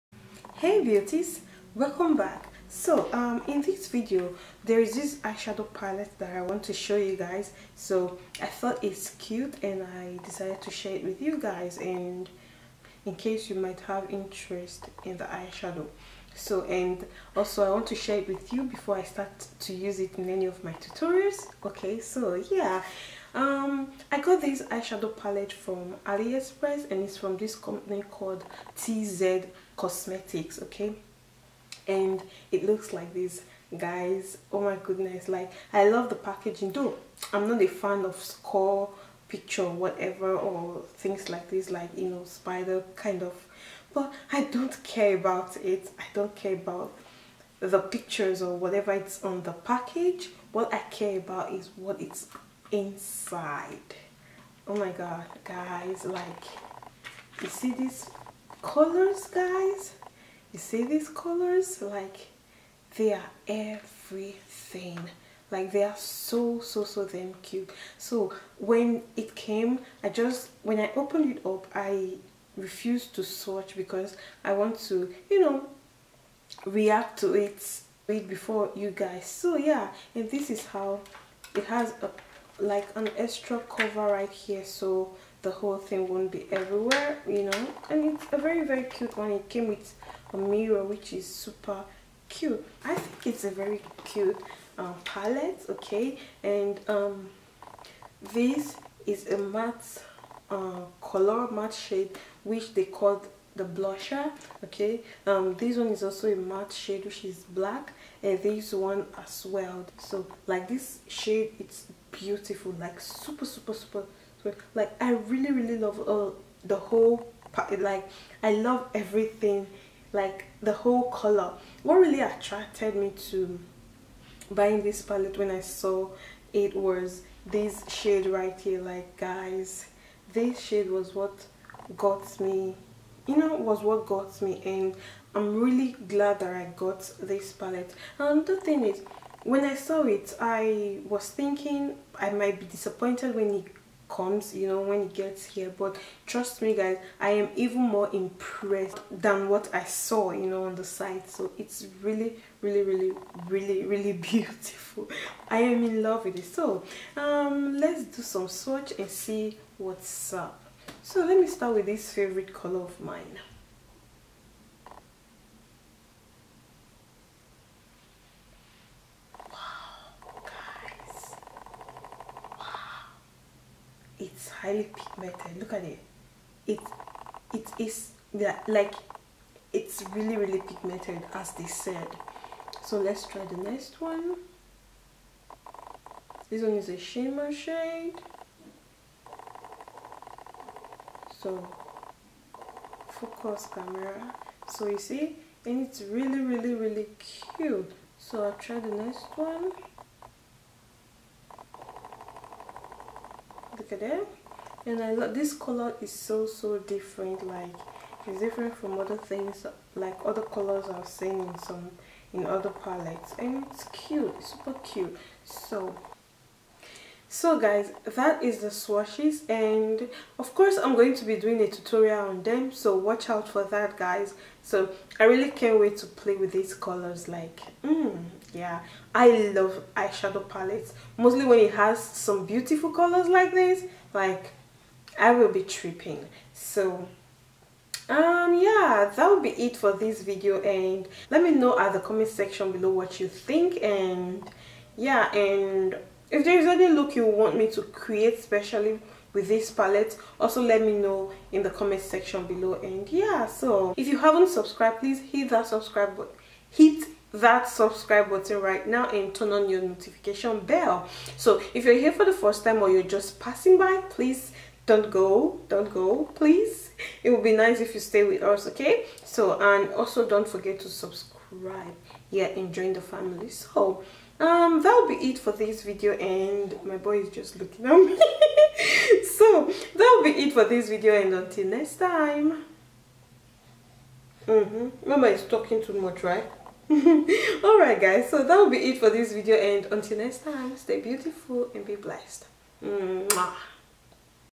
AFFORDABLE EYESHADOW PALETTE REVIEW. ALIEXPRESS.
Hello everyone, so today o will be reviewing this eye shadow palette by TZ COSMETIX. I bought this palette off Aliexpress and the delivery took two weeks.

ojocordilia25 @gmail.com

Dilias Eempire

Tweeter:
Dilia_empire

Until next time, stay beautiful and be blessed.